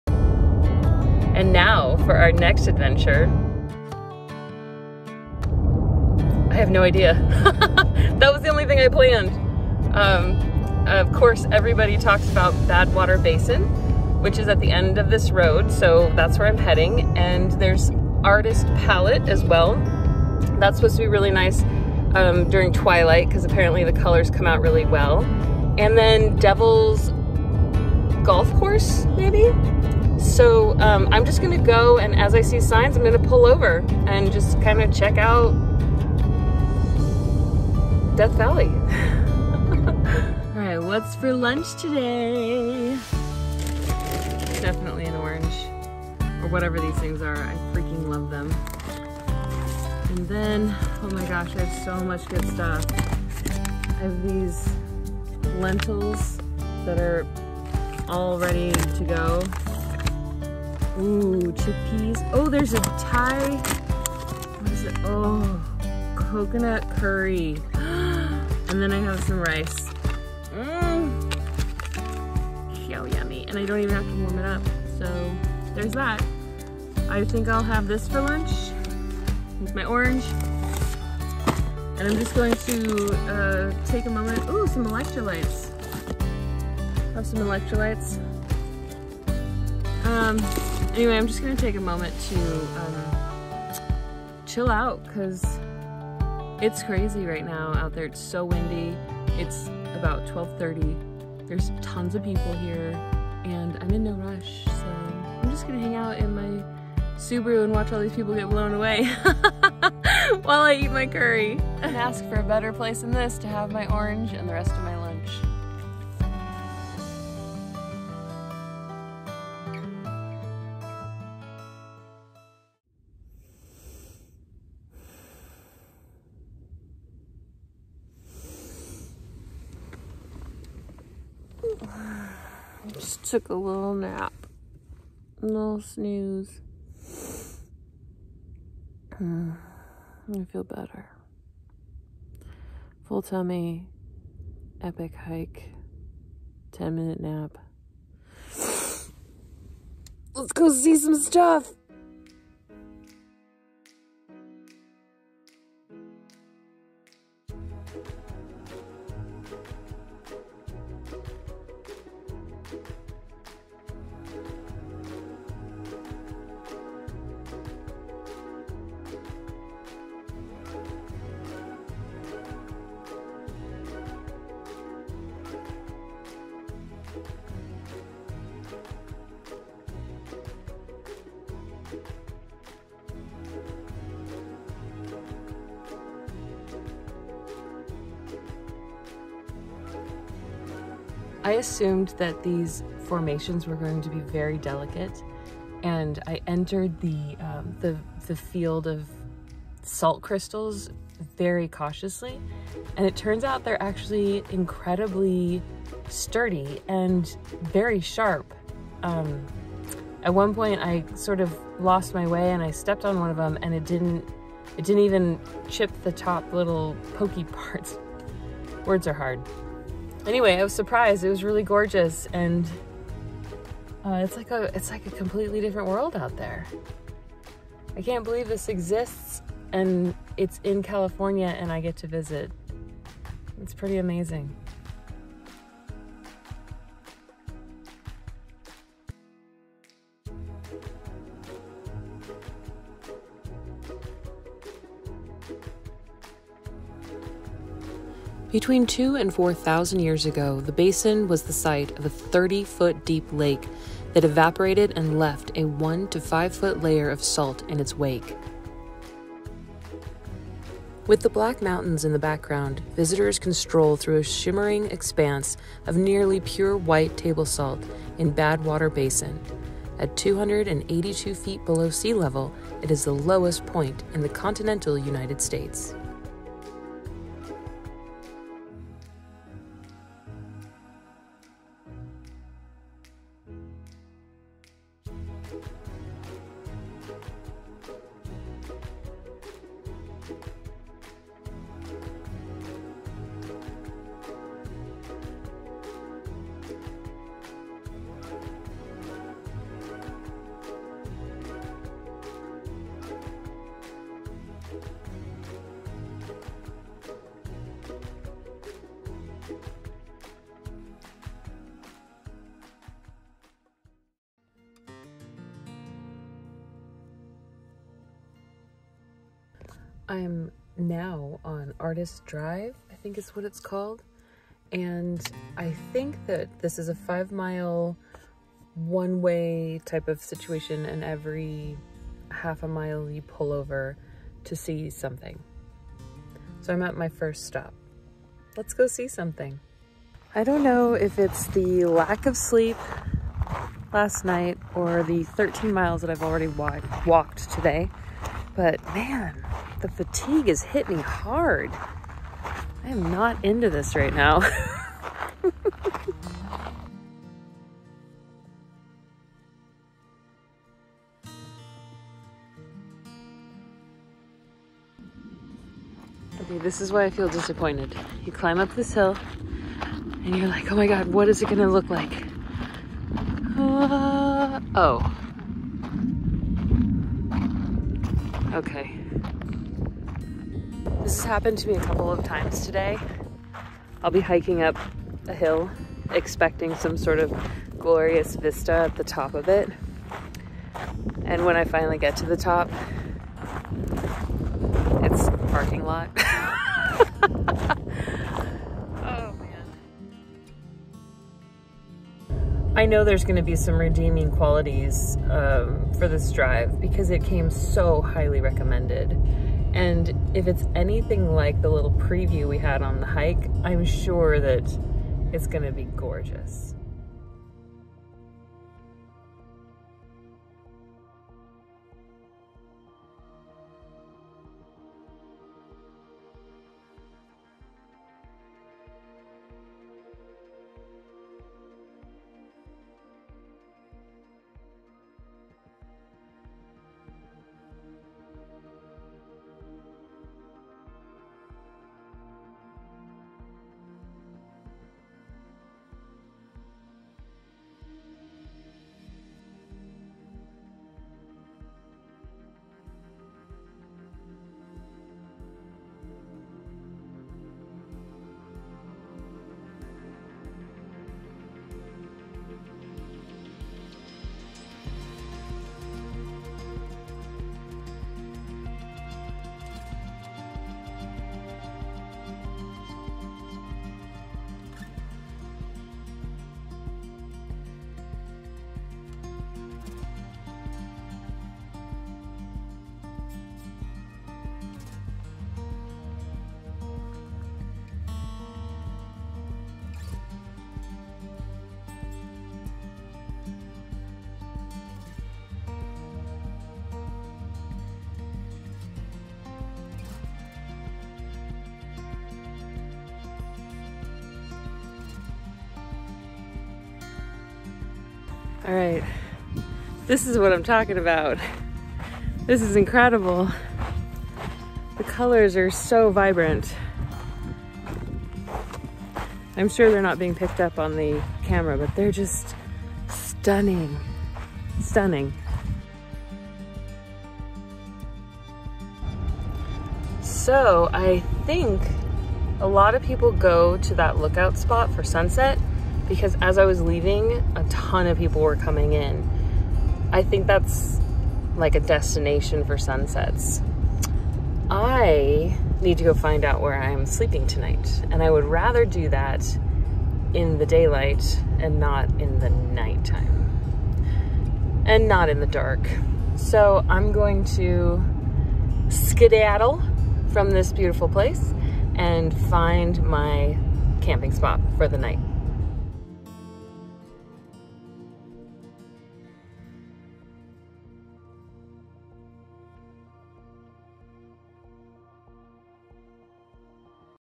Death Valley/ Day One: Part Two/ Badwater Basin
After a wonderful morning hike I made myself some lunch, took a nap and explored badwater basin, the devils golf course and the artist drive. The sunrises and sunsets in the park are some of the best I've ever seen. My first day in Death Valley was definitely amazing.